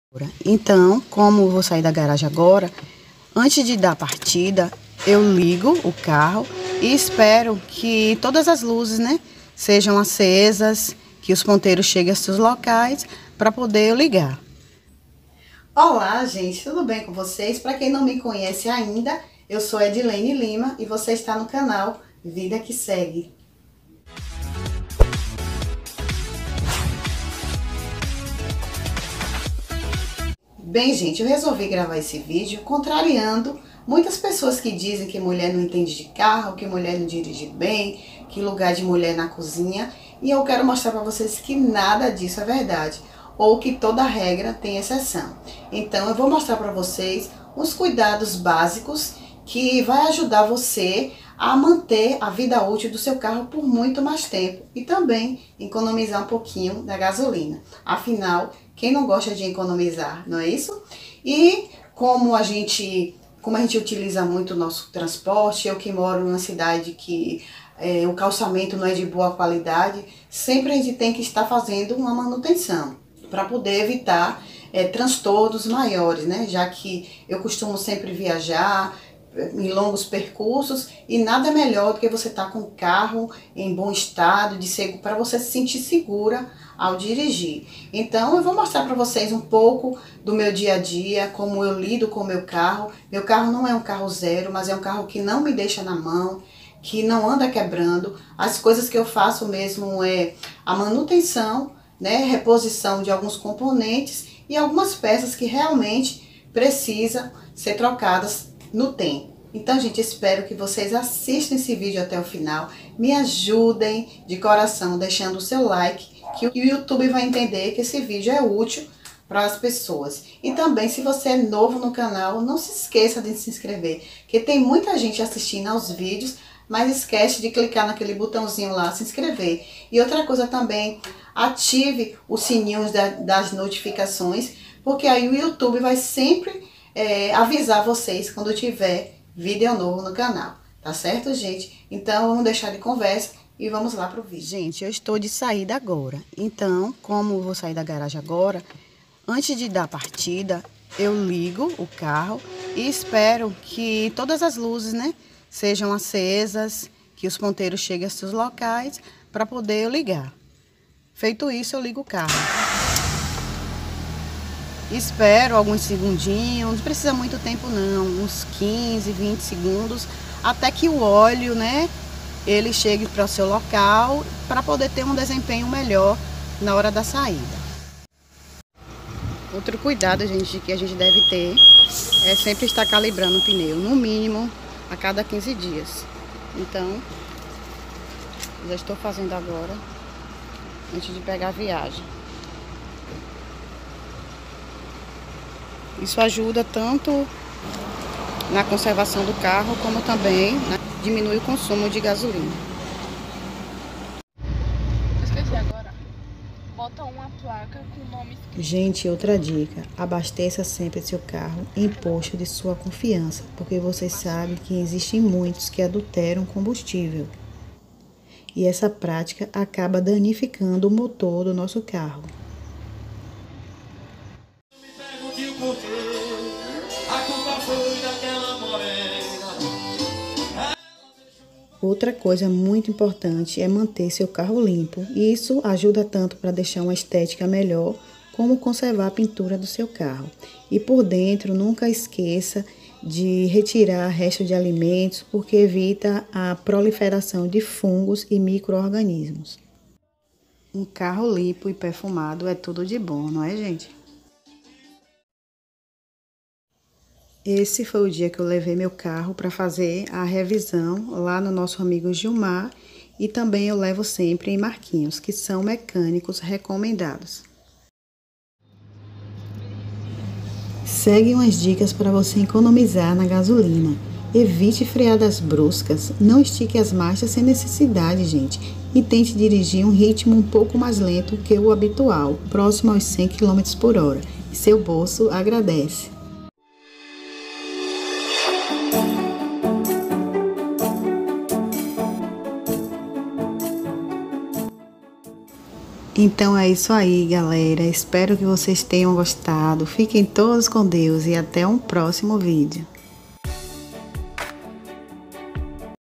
Como evitar danos no carro/economizar combustível
Será que mulher sabe cuidar do seu carro? Contrariando a muitos, segue umas dicas básicas que vão ajudar a aumentar a vida útil do seu carro e ainda economizar o combustivel..Nosso bolso agradece.

Facebook Edilene Lima Peu
cuidados com o carro
economize gasolina